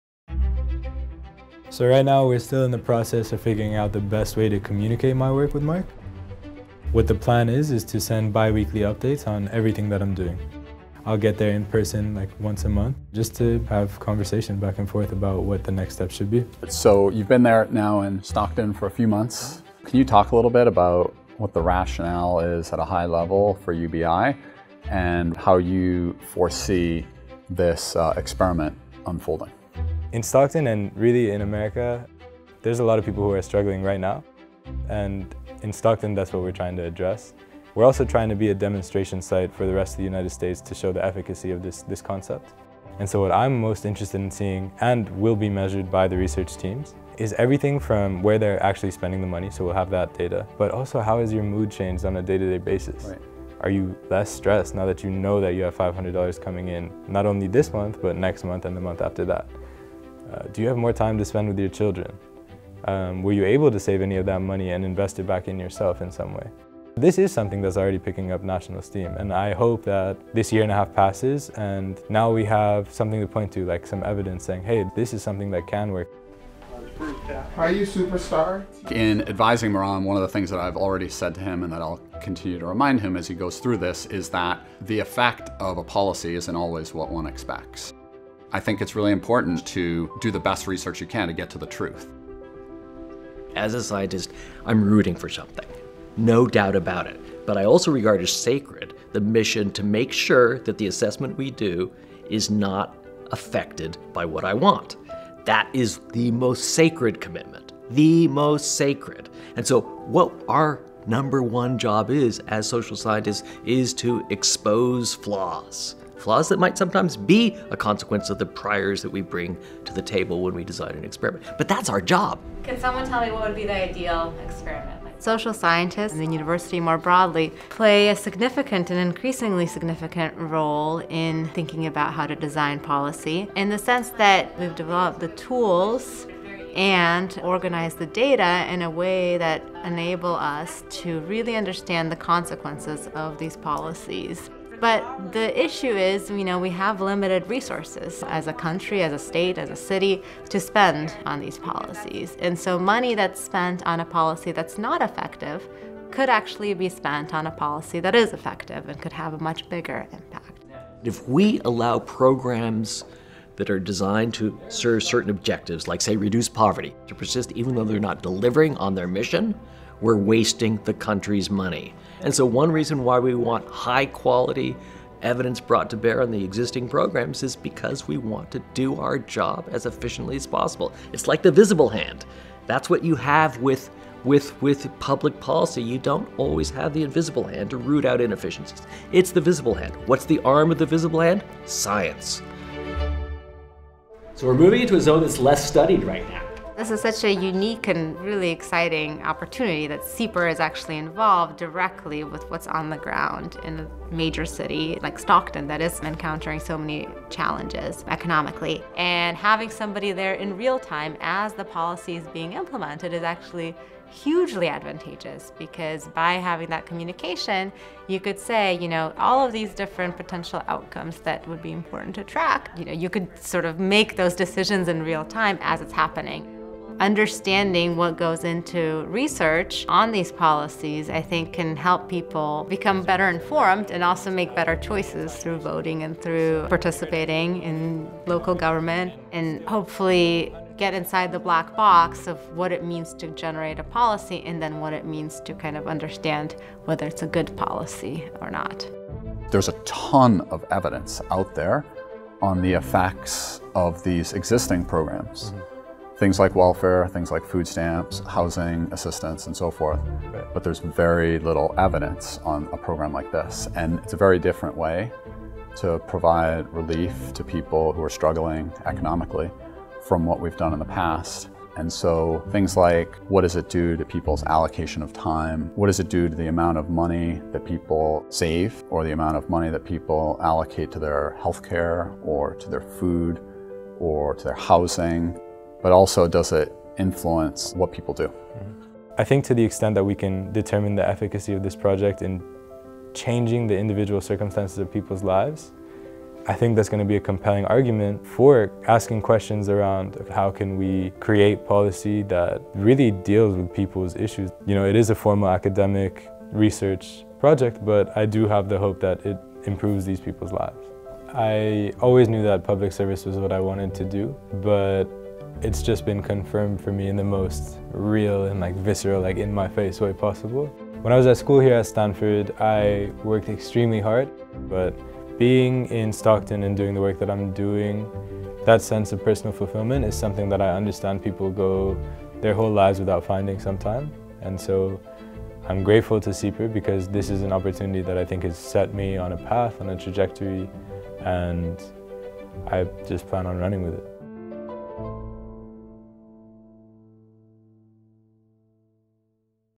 Influencing policy: "SIEPR is involved directly with what’s on the ground in a major city."
As Mehraan Keval, '18, continues his postdoctoral fellowship studying the effects of Stockton's universal basic income pilot program, he reflects on the importance of measuring the policy with SIEPR Director Mark Duggan. Professors David Grusky and Maya Rossin-Slater also discuss how social science plays a role in crafting better public policies.